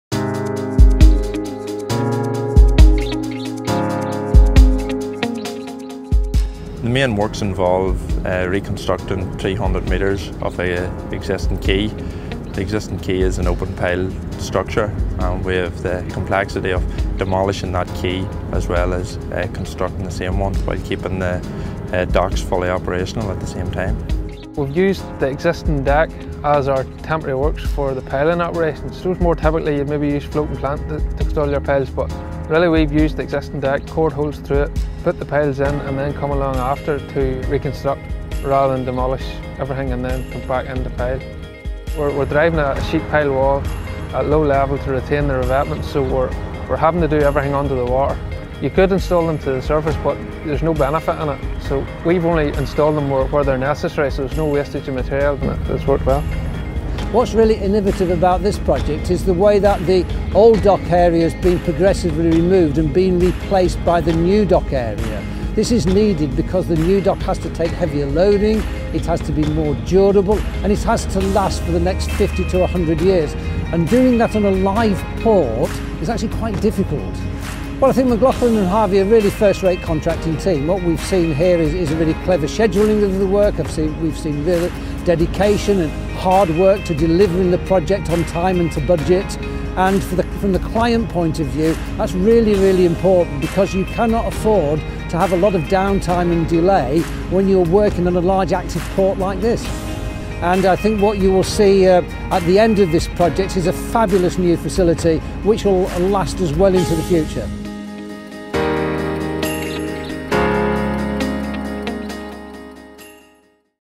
Teesport Berth 1 Reconstruction
Teesport Phase 1 Redevelopment - Banagher Precast Concrete supplied 17,658 tonnes of precast for this project, some 2,073 units including precast crossheads & pre-stressed slabs